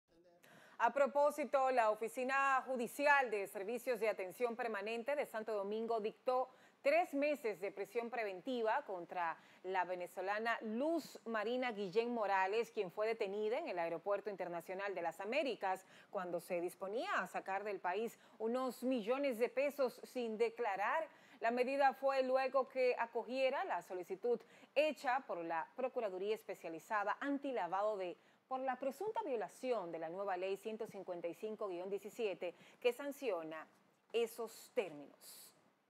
Apresan venezolano en AILA con 5 millones de pesos sin declarar
dictó tres meses de prisión preventiva contra la venezolana